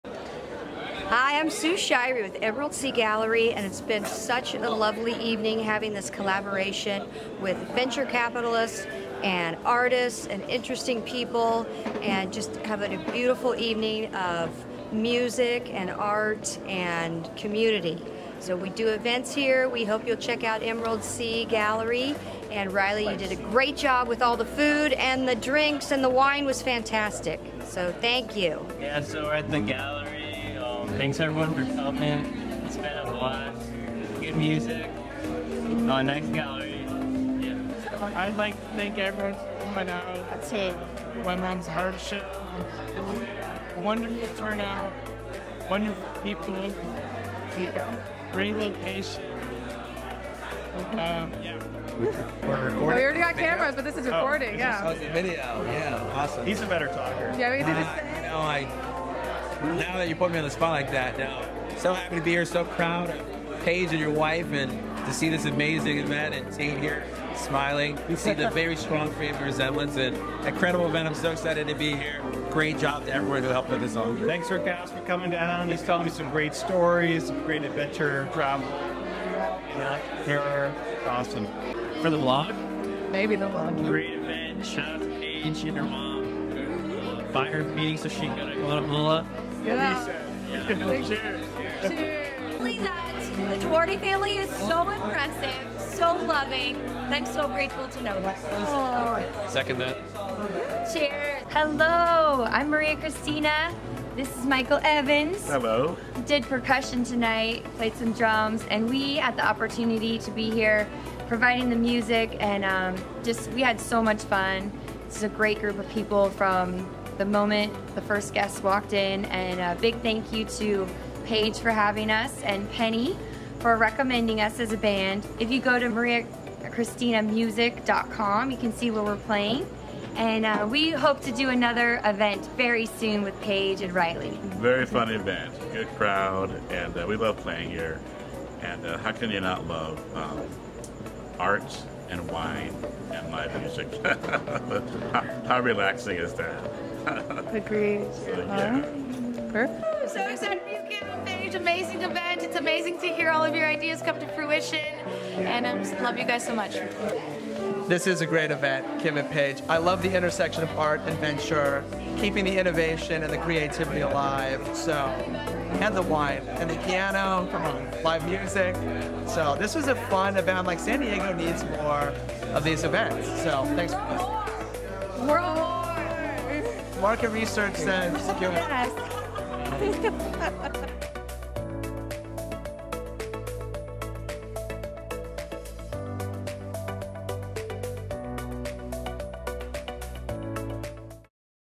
"Venture Into Art" : Intersections Between Art and Venture Capital
Venture Into Art was a collaborative event hosted by Behind Genius Ventures Founding Partner Paige Doherty, Behind Genius Ventures Chief of Staff Rylie Jennings, and Kim Doherty of Kim Doherty Art. The purpose of this event was to connect the startup & art scenes in Southern California. It was an evening full of fine art, wine by Hand of God, and live music from local band, Maria Christina, at the Emerald C Gallery in Coronado. The event highlighted fifteen original landscape oil paintings in an exhibit, “Life By the Sea Collection”.